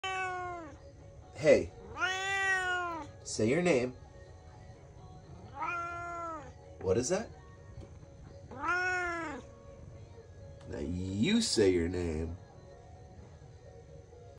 YouTube Poop
Prepare for laughter overload with this wild YouTube Poop featuring Siamese cats! 🐱😂 Watch as chaos ensues in this hilarious short clip that'll leave you in stitches! Don't forget to say your name (and hit like) if you love cat antics!